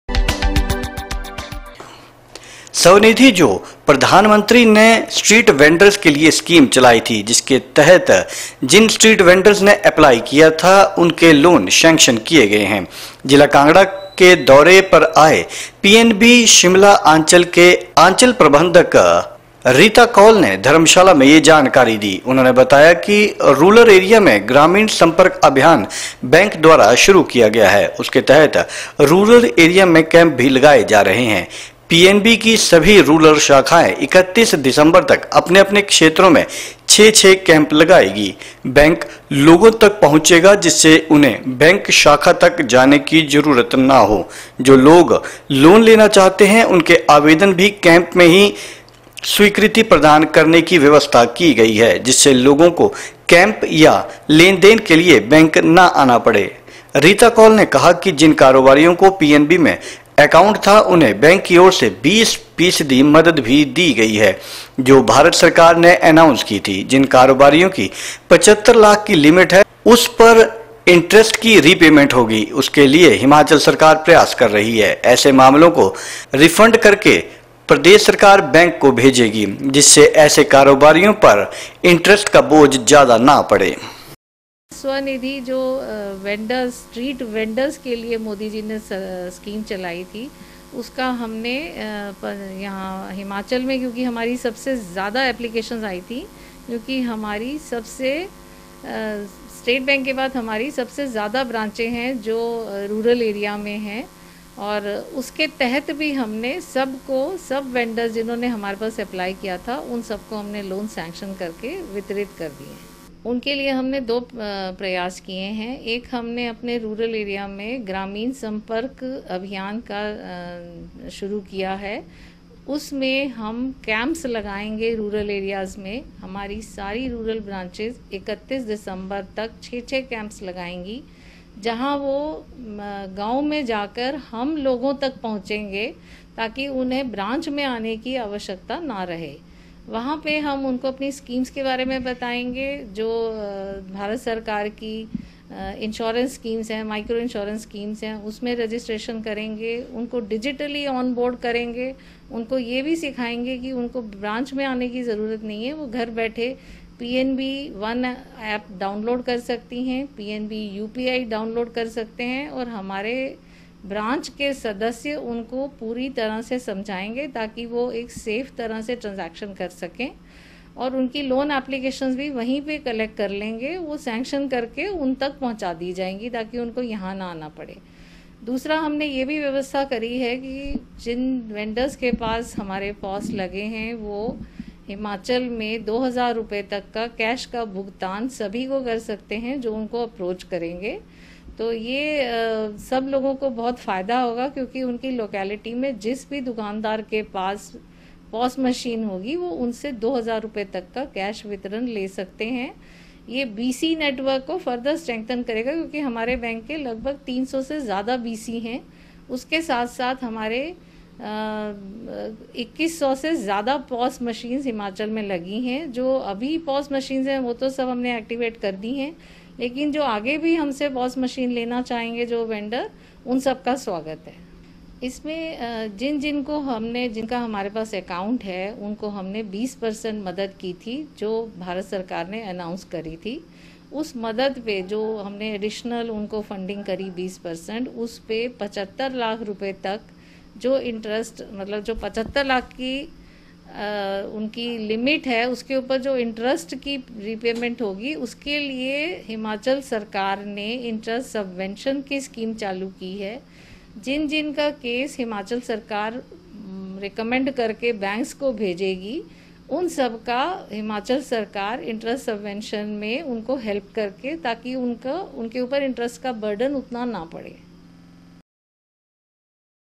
07 oct 3 स्वनिधि से आवेदन करने वाले स्ट्रीट बेंडर्स के लोन किए स्वीकृत
स्वनिधि से आवेदन करने वाले स्ट्रीट बेंडर्स के लोन किए स्वीकृत
ग्रामीण संपर्क अभियान किया गया शुरू